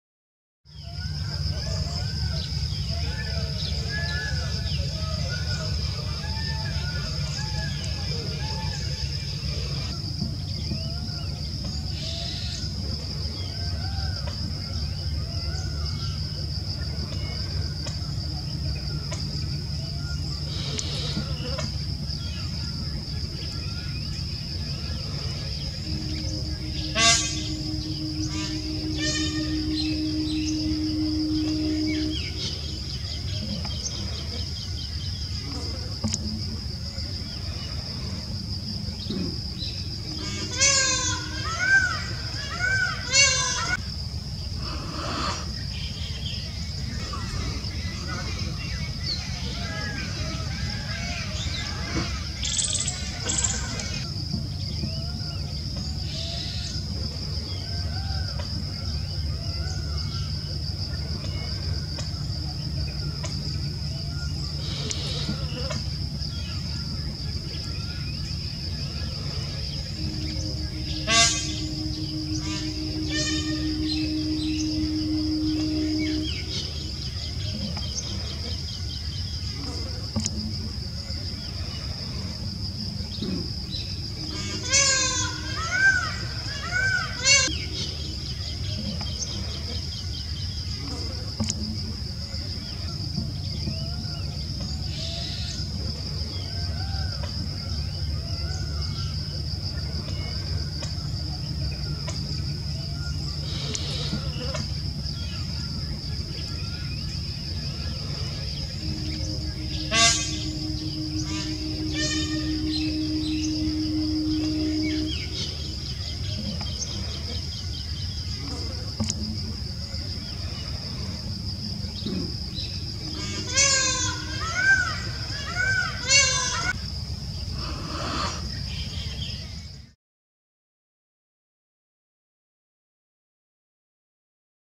Leopard rests after hunting a wholesome deer for dinner
Leopard(Panthera pardus) sits quietly on the ground after enjoying a deer in dinner. He pants while resting and probably will fall asleep soon.